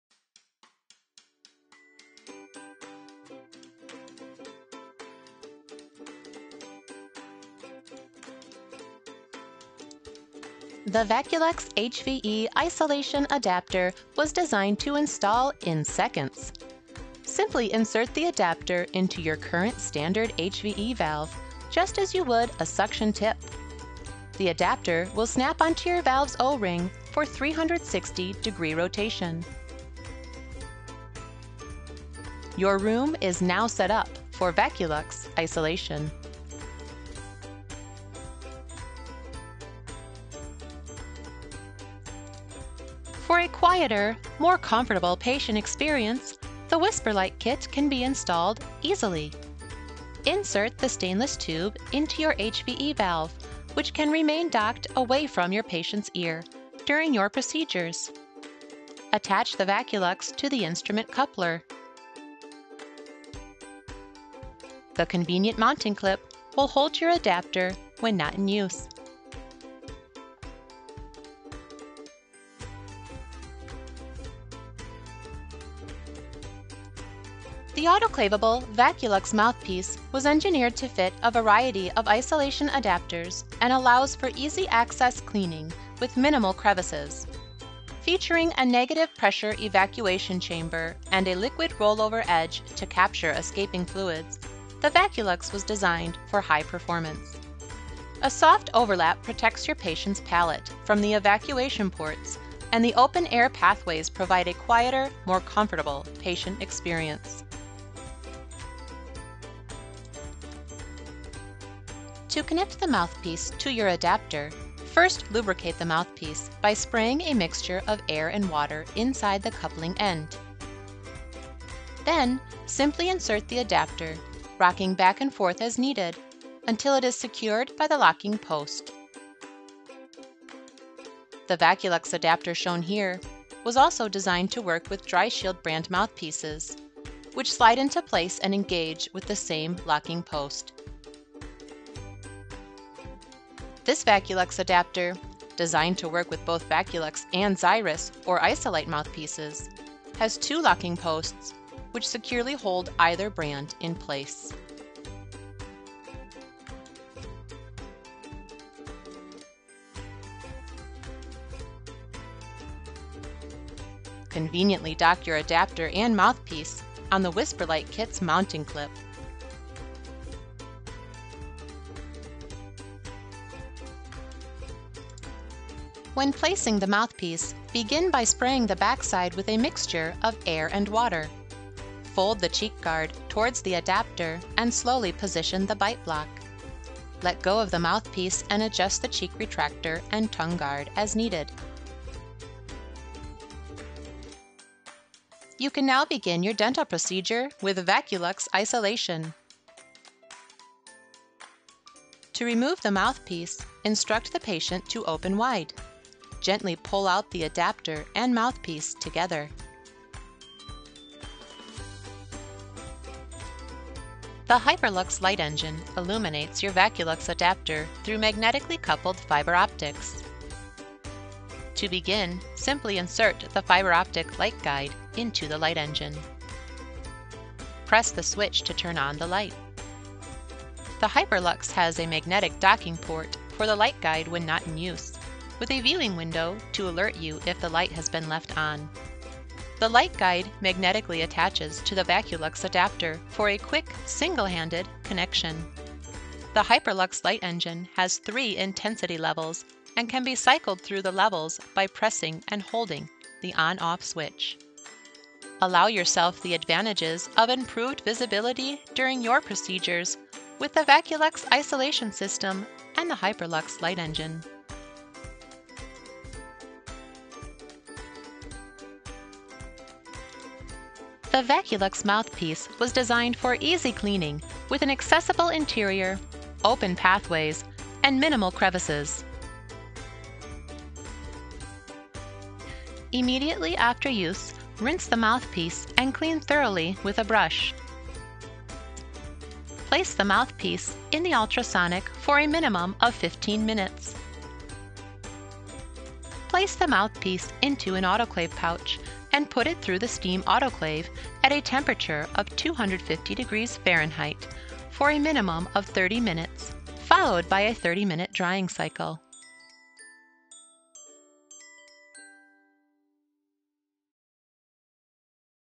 Getting Started with the VacuLUX Isolation System
A quick-start guide for the VacuLUX Isolation System. Learn installation, mouthpiece placement techniques, illumination options, sterilization and more.